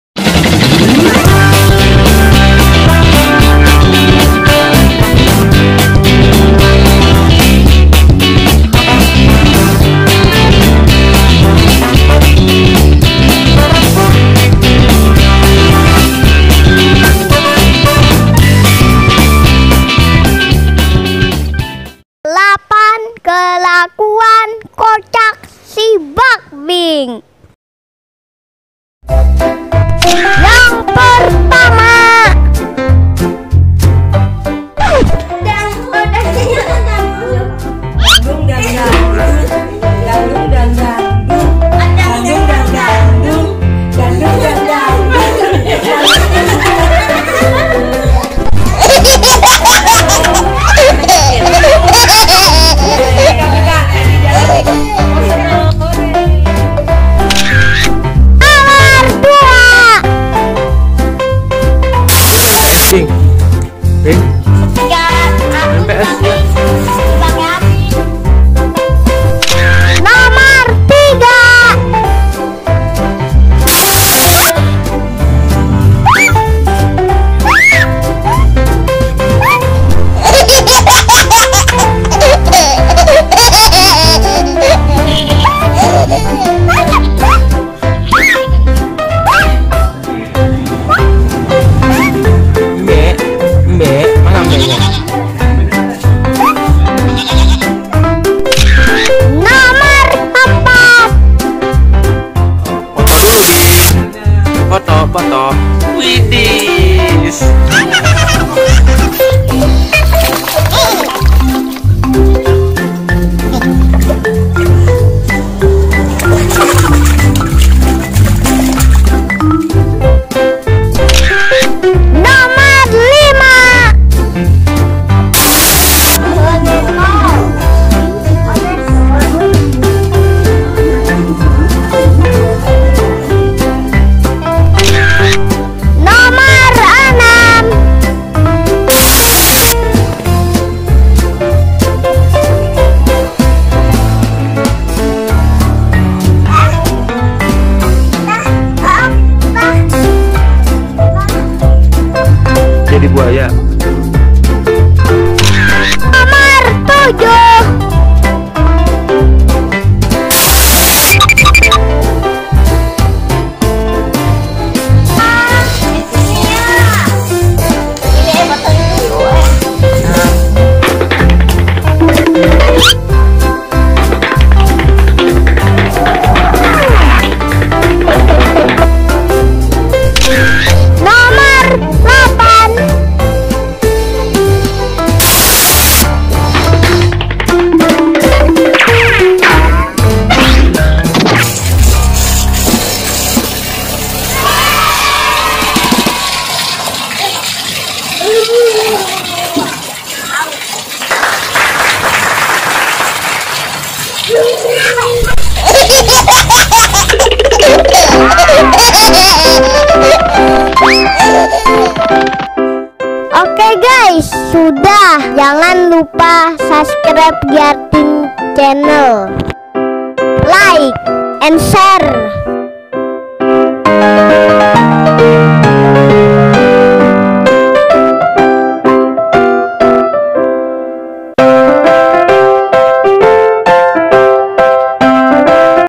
8 KELAKUAN KOCAK SI BAKBINKS
Hallo guys,
Kali ini kita akan lucu2 an bersama bakbinks anak terkecil dari Giartin family.

Terima kasih.